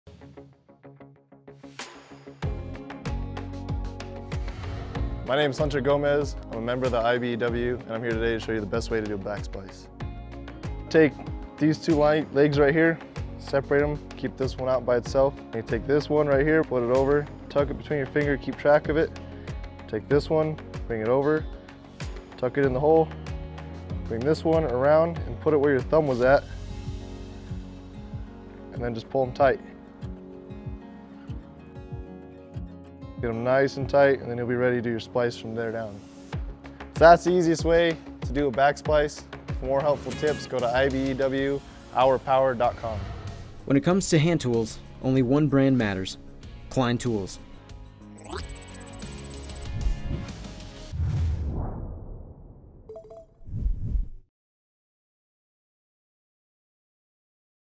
Job Tips: Best Way to Start a Back Splice
Job Tips are a quick and easy way to learn something new that will help you be more efficient while on the job.  Often times our job depends on the quality equipment we use.  This back splice tip will help you prevent fraying at the tip of your rope in whatever situation you’re in.